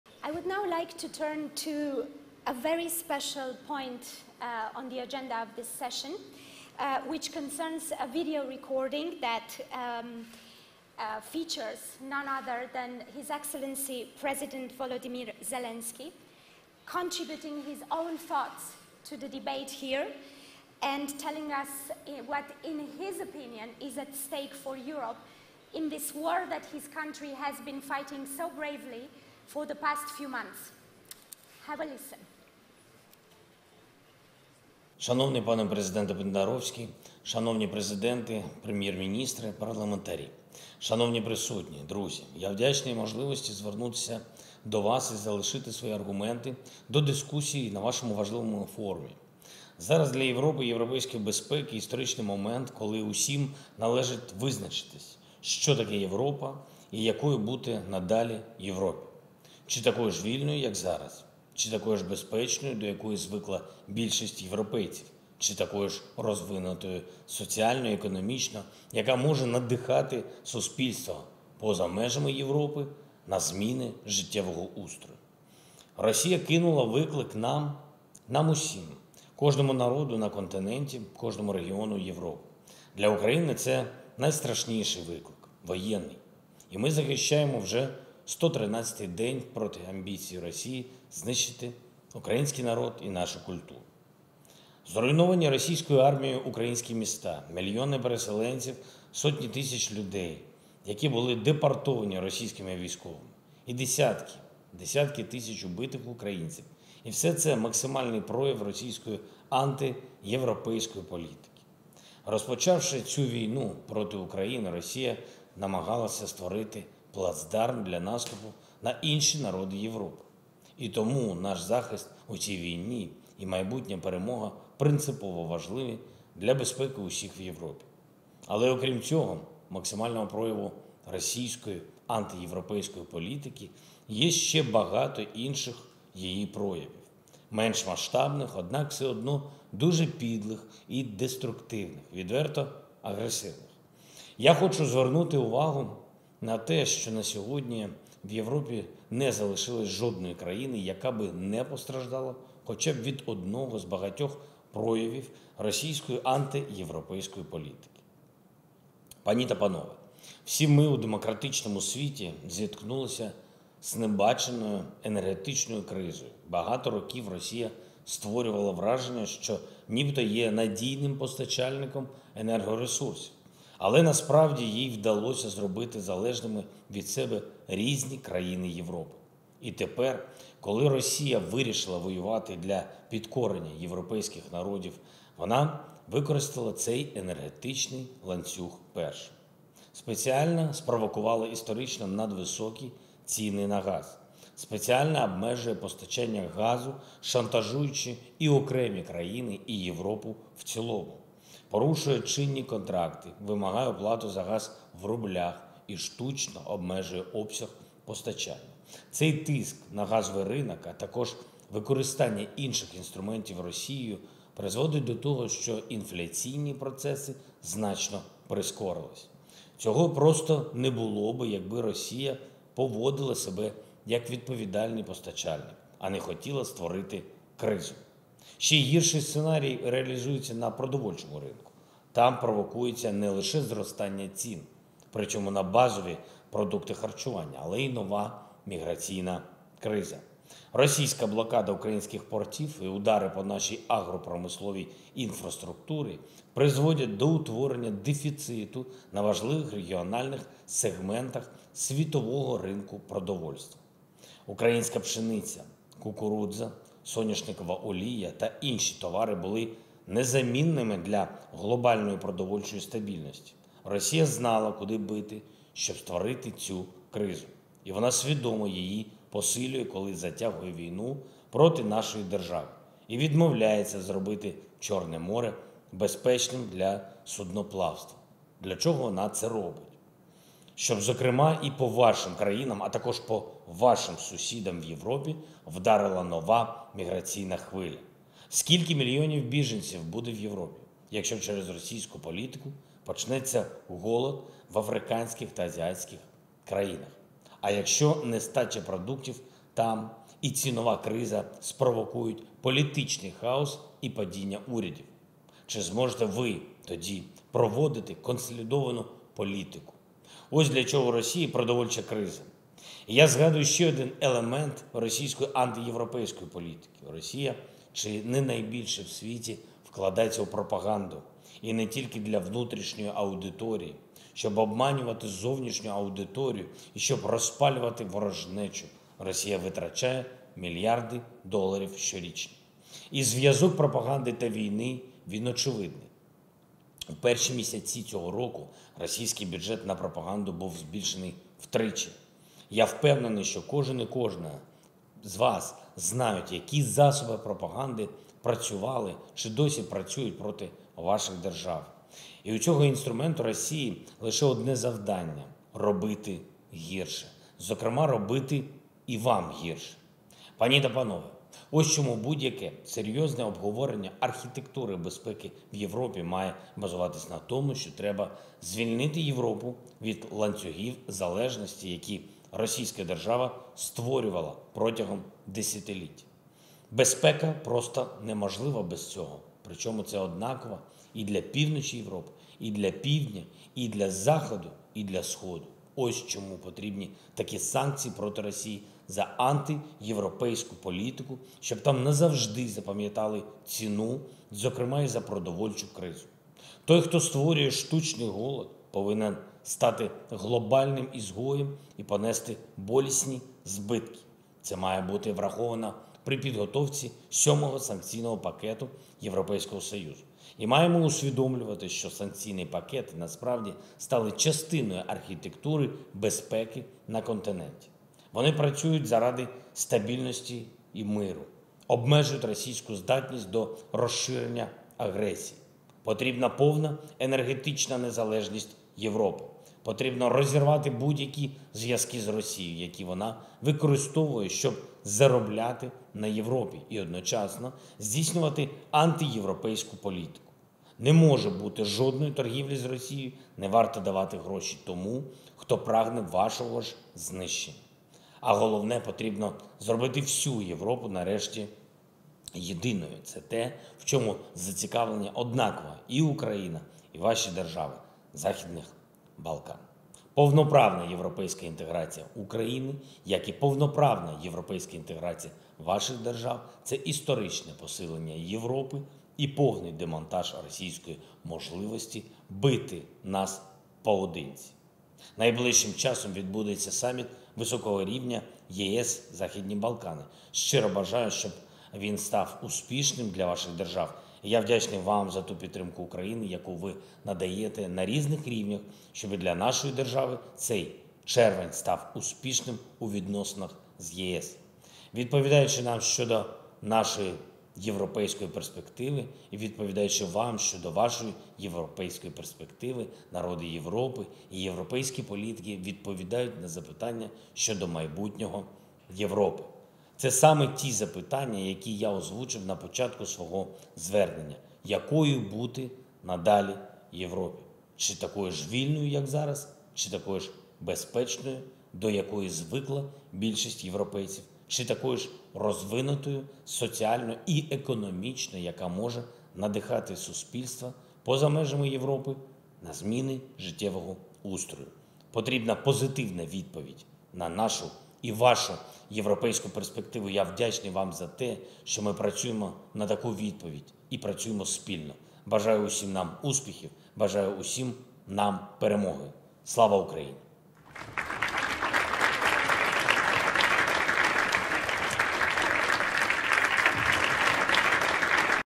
Володимир Зеленський звернувся до учасників Діалогу Преспанського форуму
«Будь-яке серйозне обговорення архітектури безпеки у Європі має базуватися на тому, що треба звільнити Європу від ланцюгів залежності, які російська держава створювала протягом десятиліть. Безпека просто неможлива без цього, причому це однаково і для півночі Європи, і для півдня, і для заходу, і для сходу. Ось чому потрібні такі санкції проти Росії за антиєвропейську політику, щоб там назавжди запамʼятали ціну, зокрема й за продовольчу кризу. Той, хто створює штучний голод, повинен стати глобальним ізгоєм і зазнати болісних збитків. Це має бути враховано при підготовці сьомого санкційного пакету ЄС».